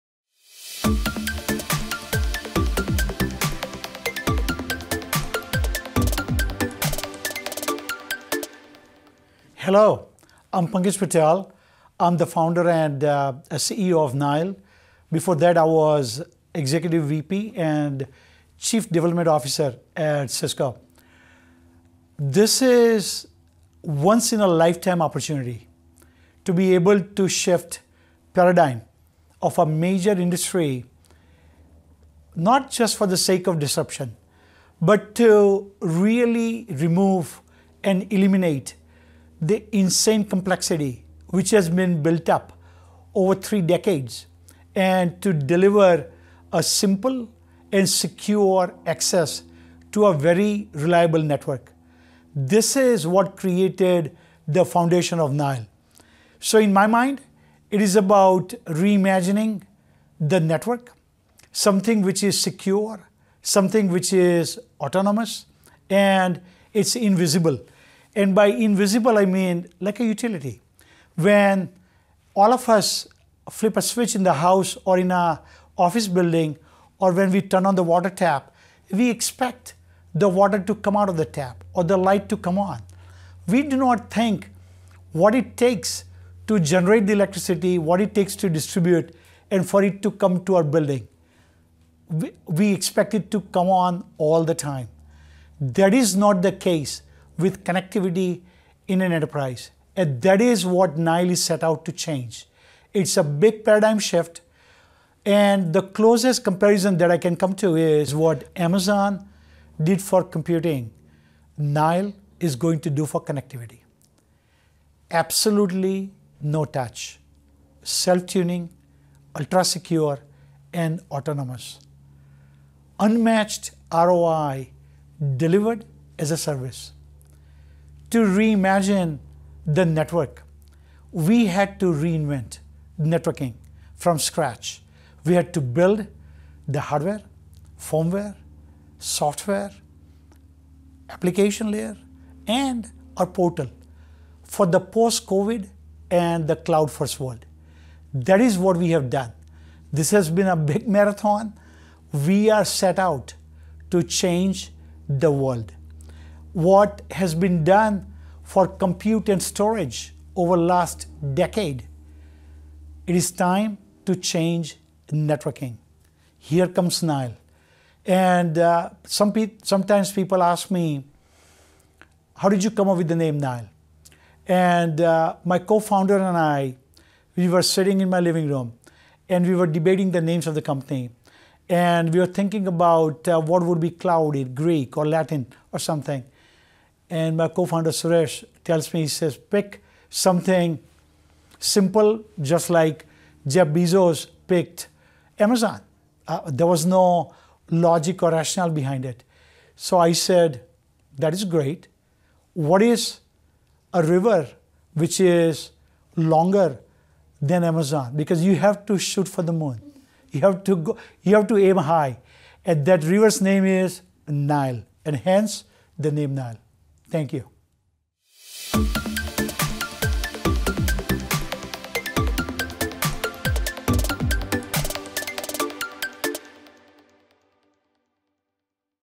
CEO Pankaj Patel | Disrupting the Networking Industry
Former Cisco EVP and CDO, Pankaj Patel is taking a once in a lifetime opportunity to completely shift the paradigm of the entire EnterpriseNetwork. It is about eliminating accumulated network complexities and delivering a network that is inherently Secure and simple. Hear more from Pankaj Patel and what Nile is set out to change when it comes to the future of secure connectivity.